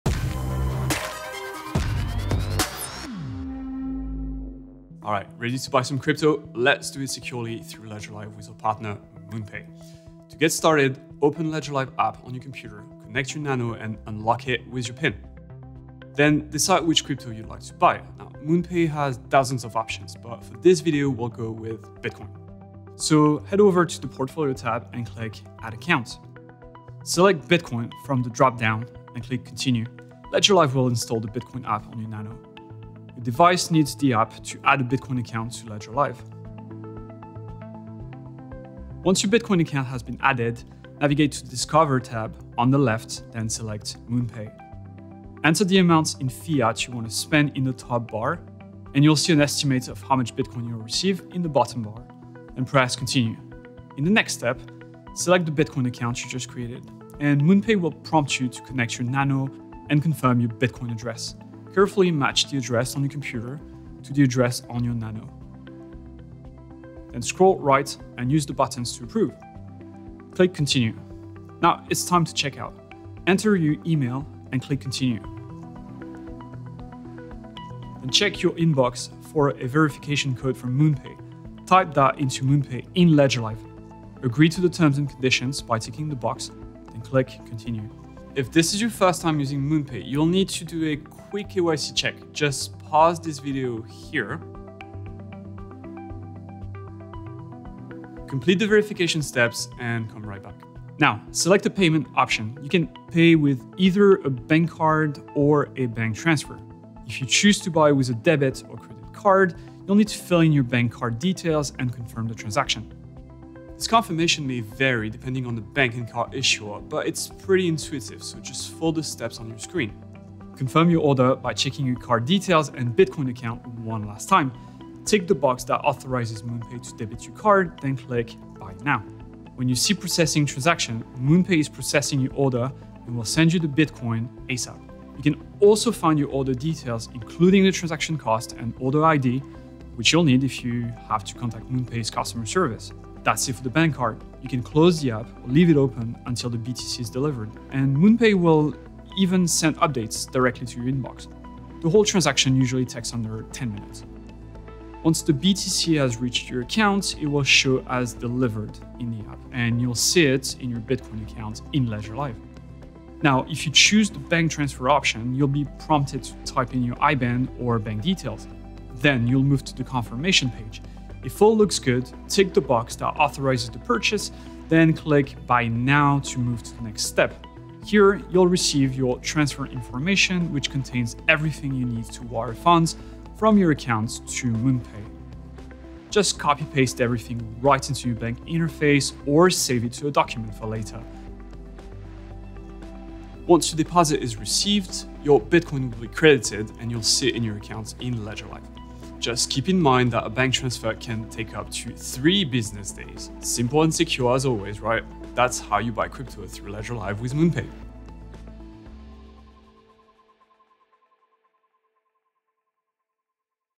Как покупать криптовалюту через Ledger с помощью MoonPay
Теперь вы можете покупать множество криптовалют непосредственно в приложении Ledger Live благодаря нашему партнёру – Moonpay. Узнайте, как это сделать, в нашем обучающем видеоролике!